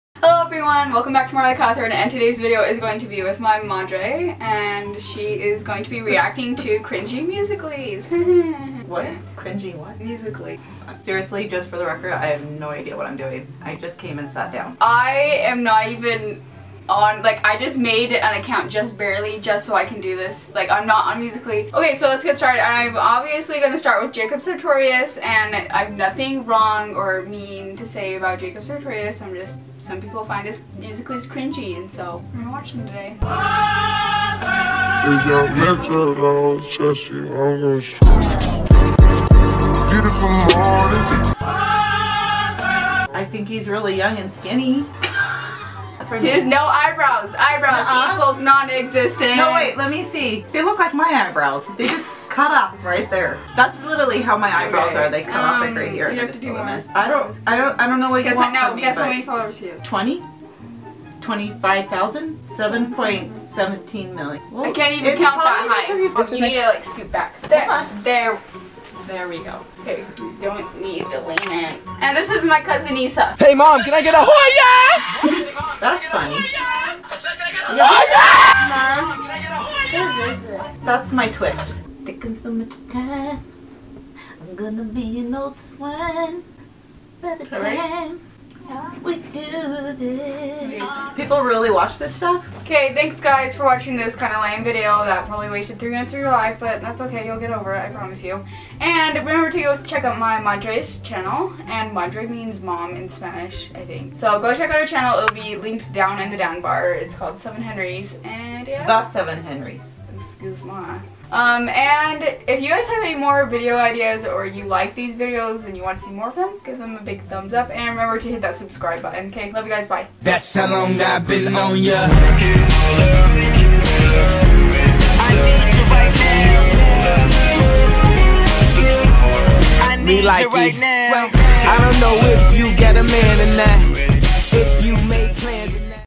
MOM REACTS TO CRINGEY MUSICAL.LYS!
Reacting to cringey musical.lys featuring my mom! This video was super short and was meant to be more fun than anything haha!